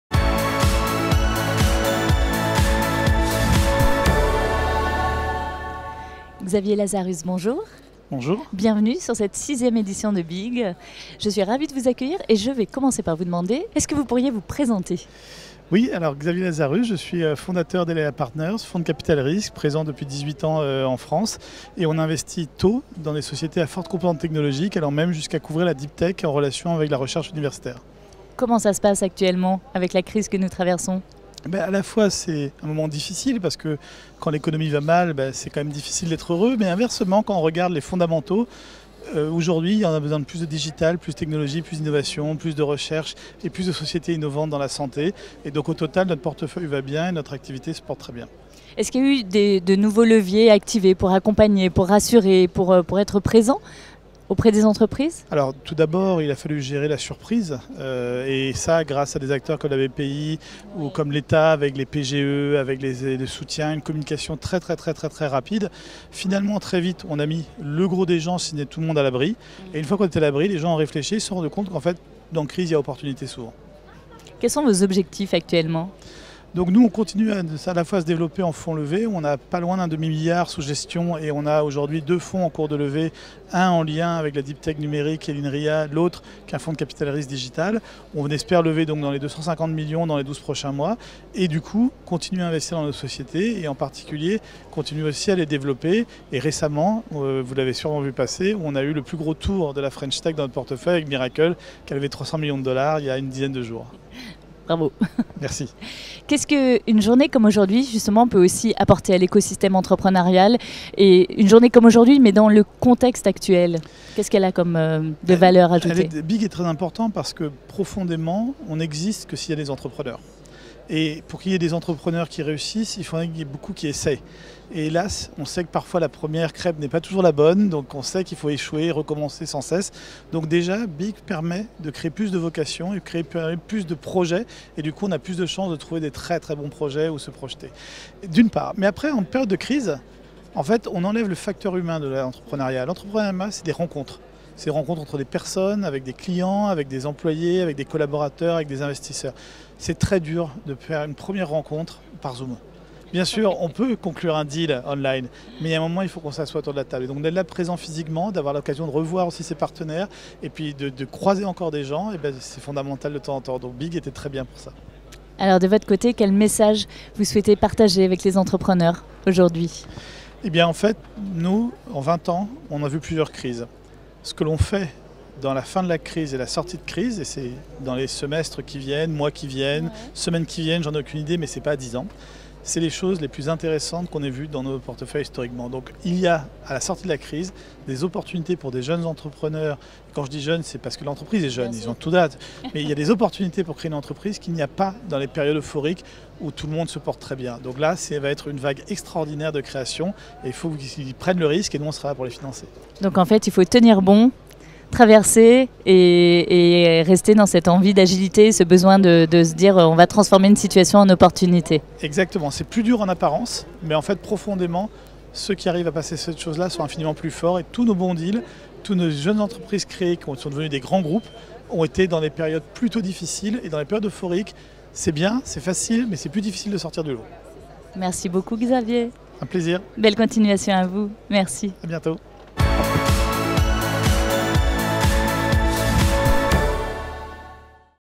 Le Video call de Xavier Lazarus, co-fondateur d'Elaia Partners
Le Vidéocall de Big2020 met en avant l’écosystème entrepreneurial français. Xavier Lazarus, co-fondateur d'Elaia Partners, répond aux questions de Bpifrance à l'occasion de la 6e édition de Bpifrance Inno Génération.